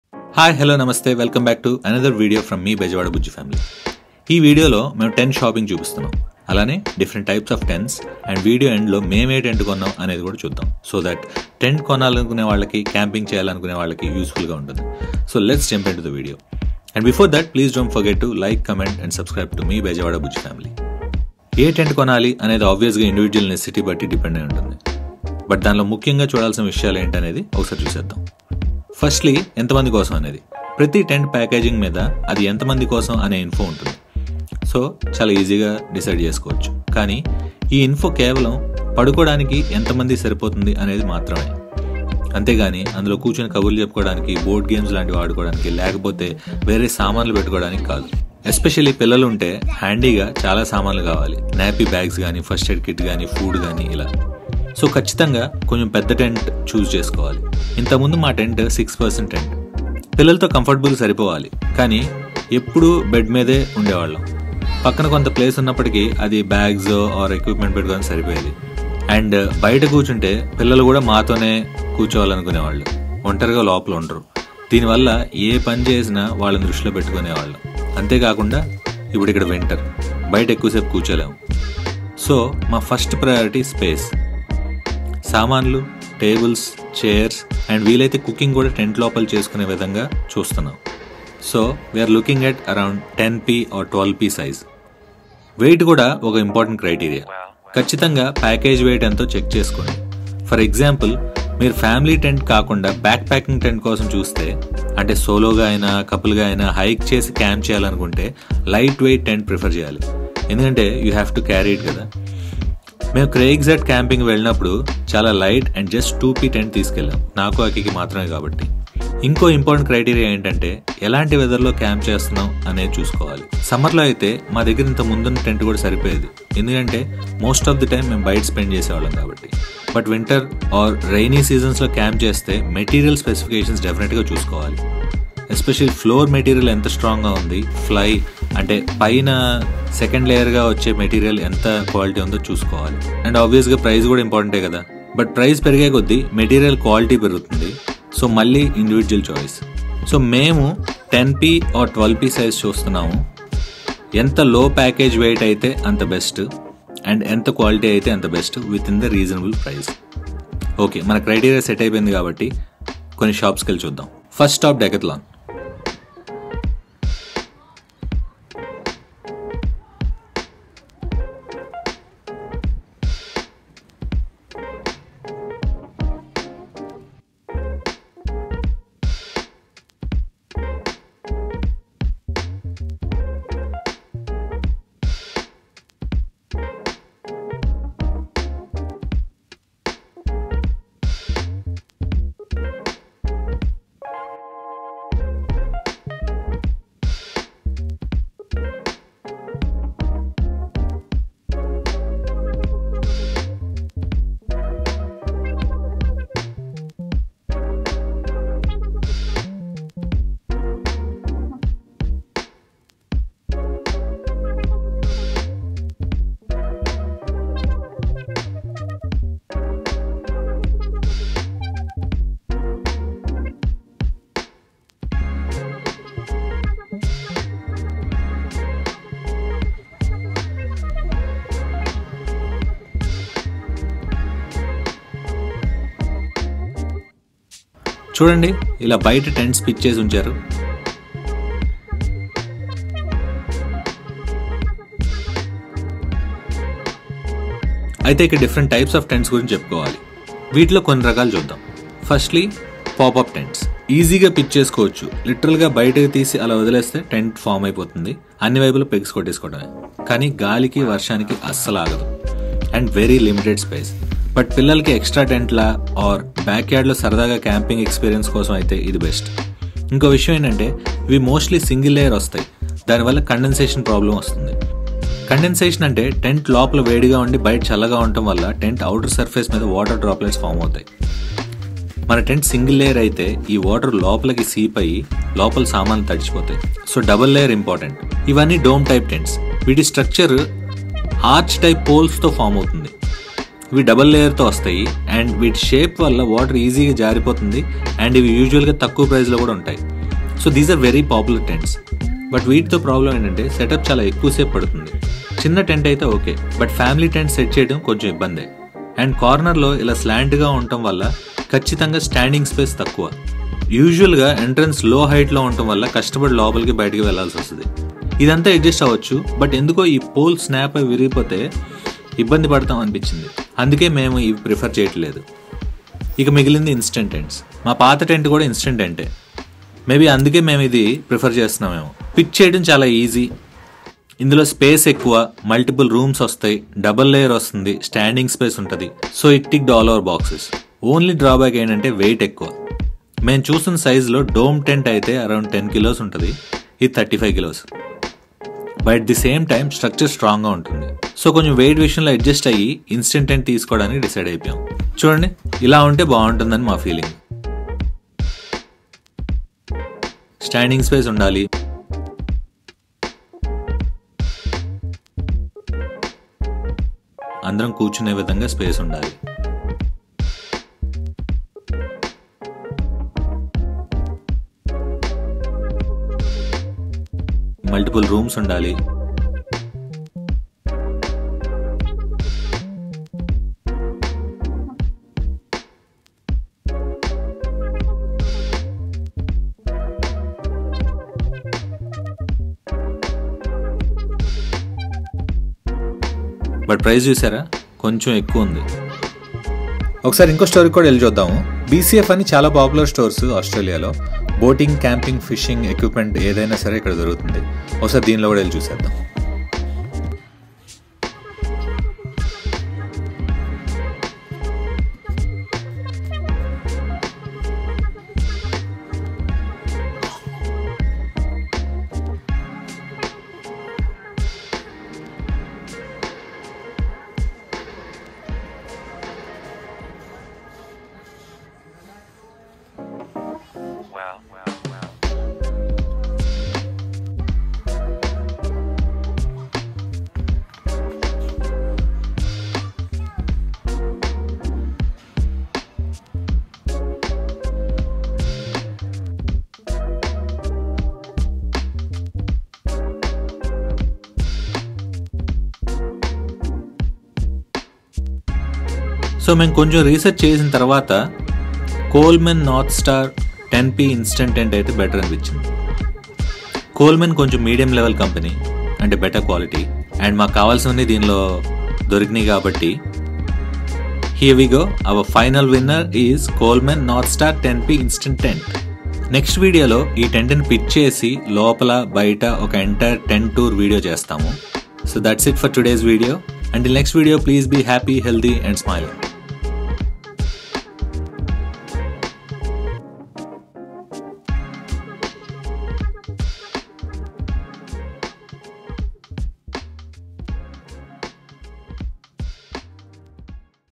shopping youtube II teluguvlogs decathlon bcf coleman anaconda melbourne camping
In this video, we are vlogging about how we chose our tent and what tent we bought. Extremely useful for beginners of camping and in general anyione who are looking to buy their tent.